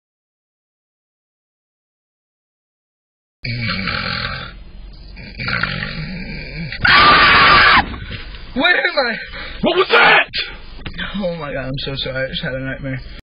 If the upper moons had a sleepover ll KNY ll my AU ll ft some ships ll 🌸Nezuko-Chaaaan (Emi)🌸
The way Akaza looked at Douma when he screamed tho😭😂

Characters :)

Kokushibo
Douma
Akaza
Hantengu
Nakime
Gyokko
Gyutaro
Daki
Kaigaku